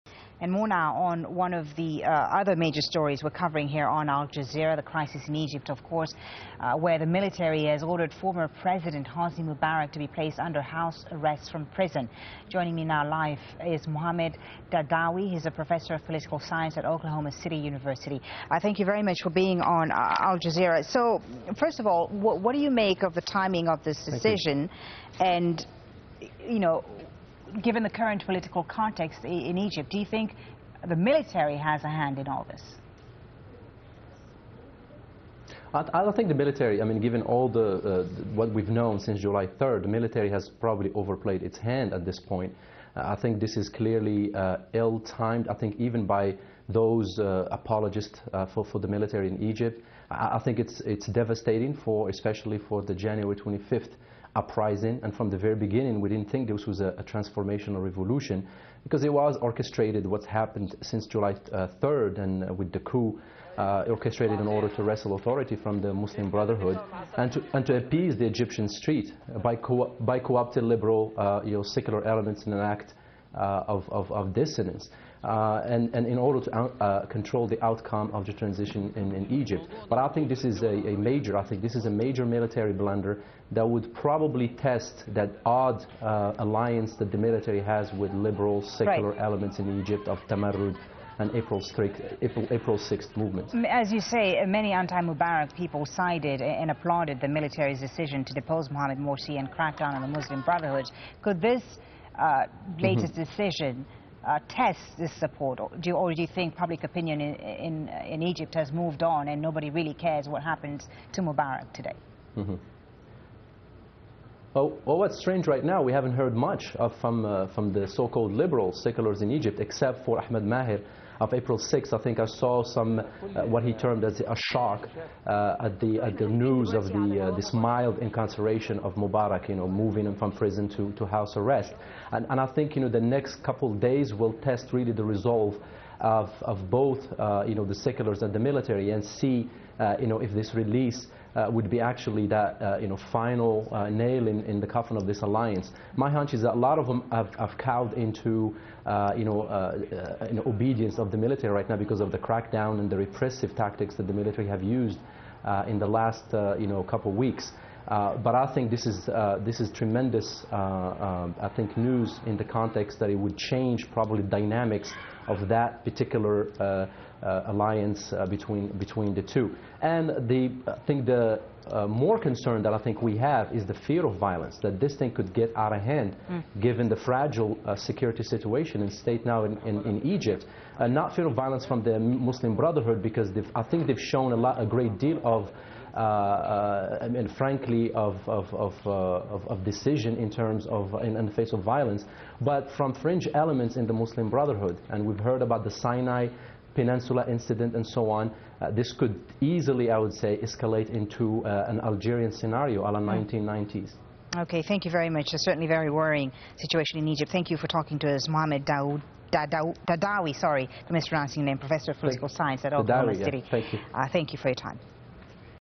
Professor Daadaoui interview with Al Jazeera
Oklahoma City University political science professor Mohamed Daadaoui was interviewed Aug. 21 about the political turmoil in Egypt.